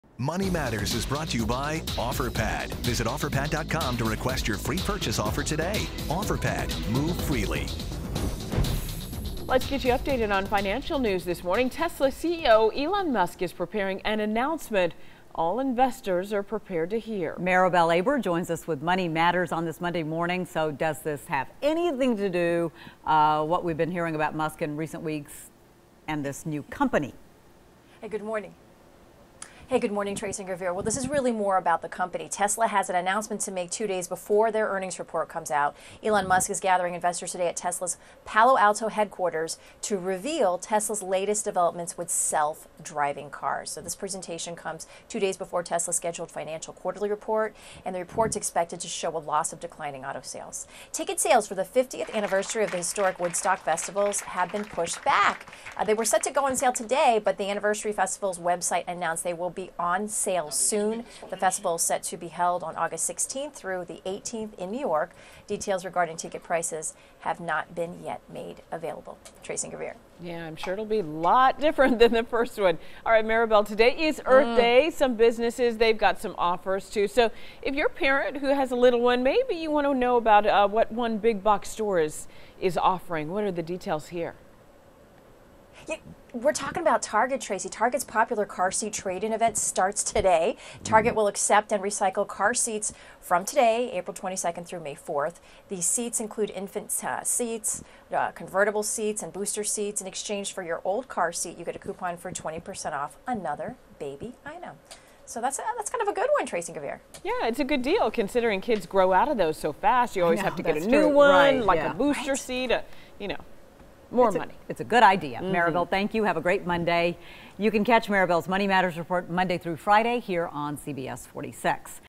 Money Matters 4/22: Tesla to make announcement 2 days before earnings report
Money Matters